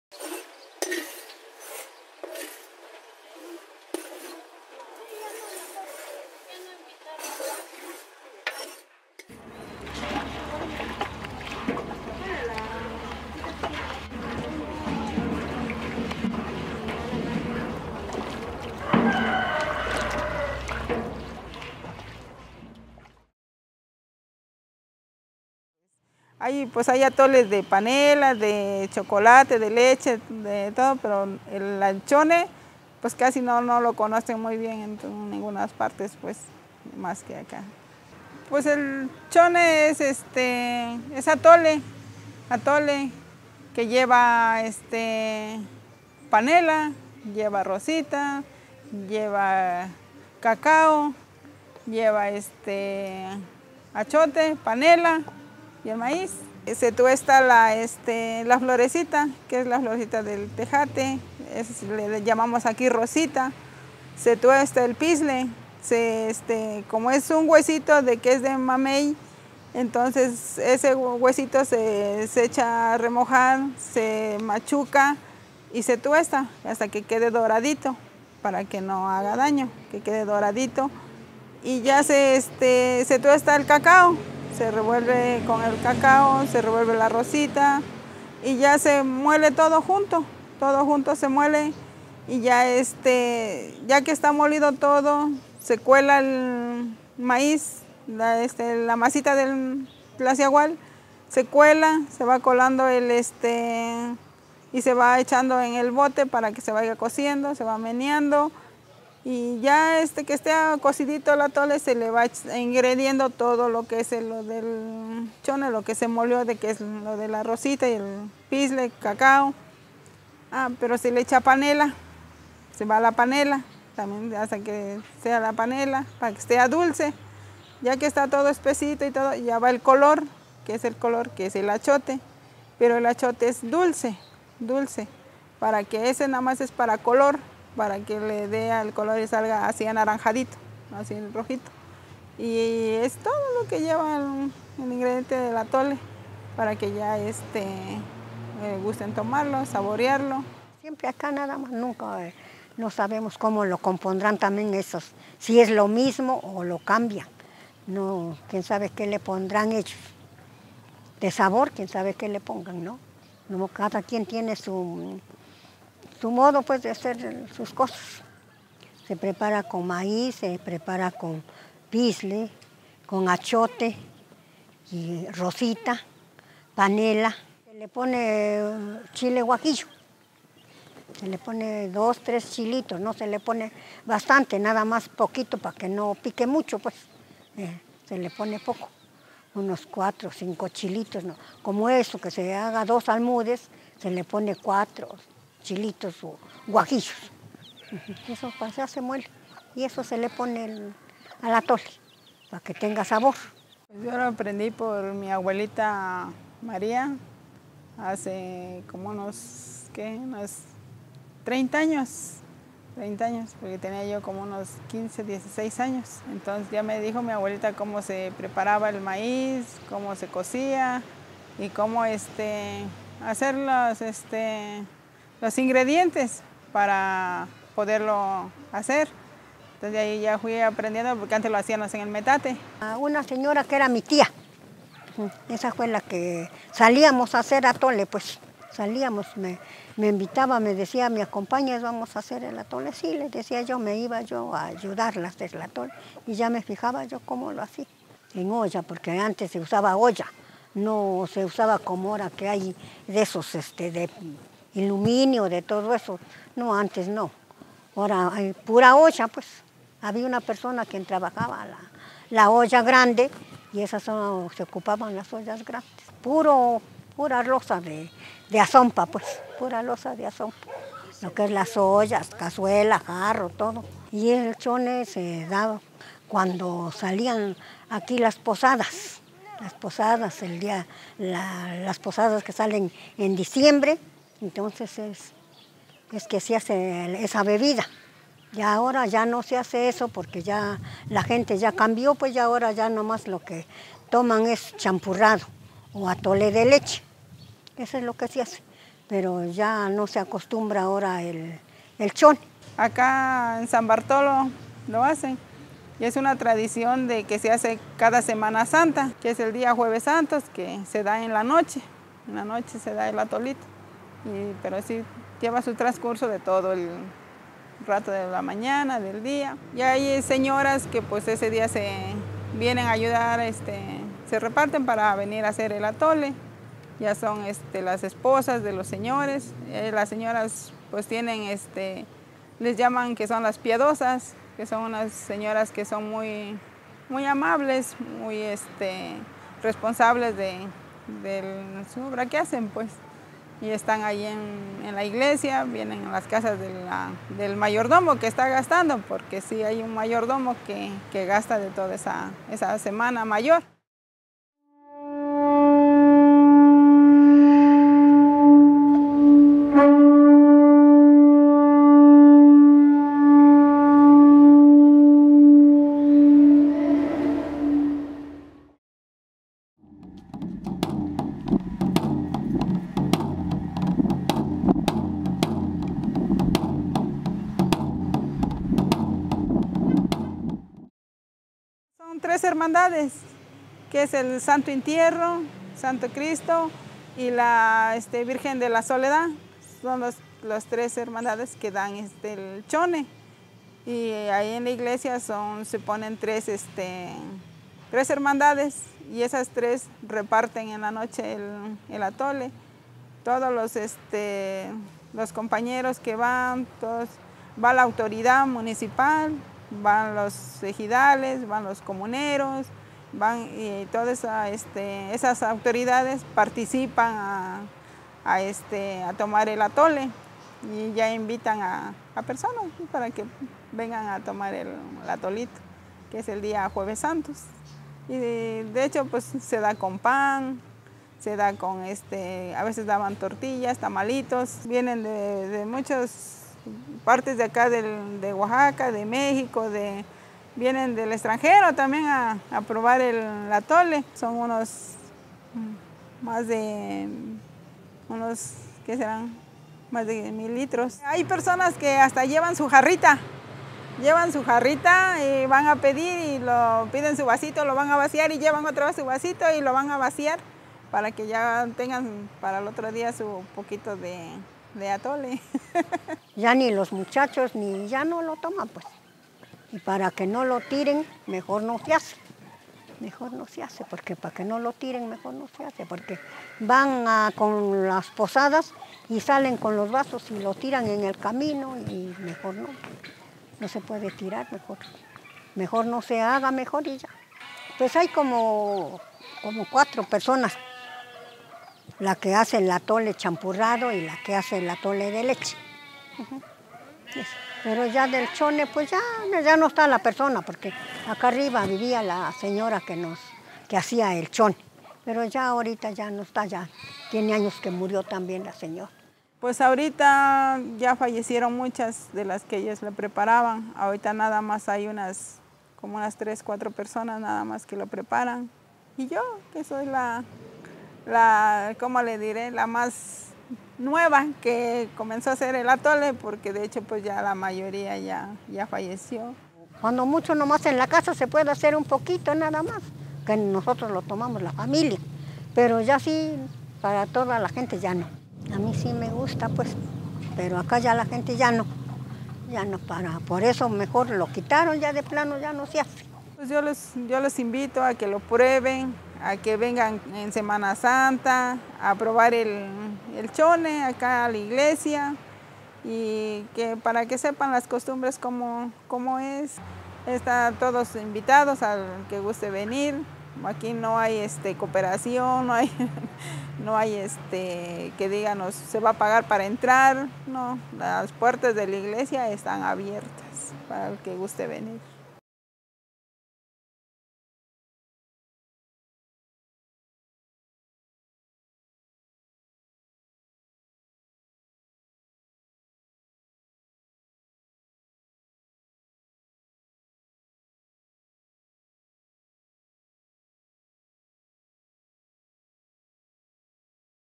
El Chone de Coyotepec y Atzompa - Bebida de Cacao
En el marco de la Posada del Cacao 2014 se destacó las bebidas de Chone de San Bartolo Coyotepec y Santa María Atzompa con una expo especial.  Este video forma parte de la expo.   Evento patrocinado por la Fundación Alfredo Harp Helú Oaxaca, A.C., la Biblioteca de Investigación Juan de Córdova y Chocolate Mayordomo.